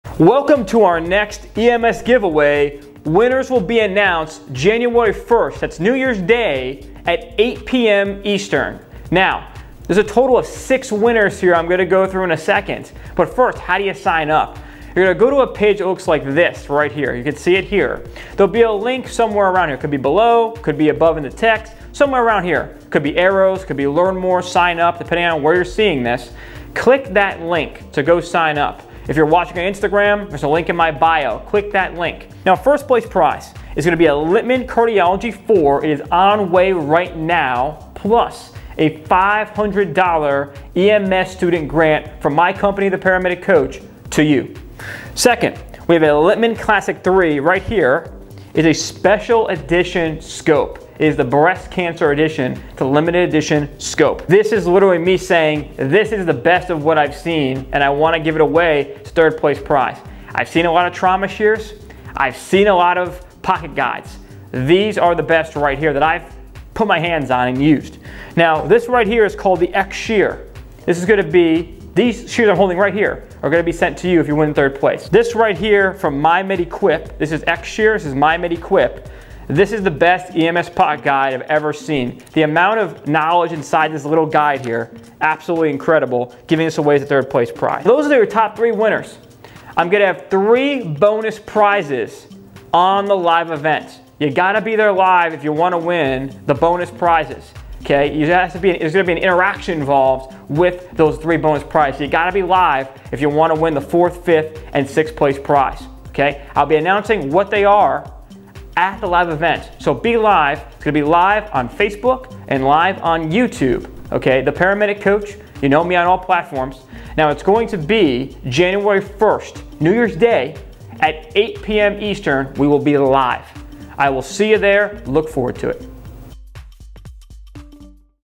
EMS Giveaway | The Paramedic Coach™ (EMTs,AEMTs,Paramedics)
First Place Prize: Five Hundred Dollar EMS Educational Grant from The Paramedic Coach™ & a Littman Cardiology IV.

Second Place Prize: Limited Edition Littman Classic III (Breast Cancer Edition)

Third Place Prize: X Shears Trauma Shears + EMS Pocket Guide from My Medi Equip.

There will be three BONUS winners at the live event, prizes and winners will be announced on January 1st, 2021 at 8pm Eastern LIVE on YouTube!

Watch even more videos from The Paramedic Coach® below;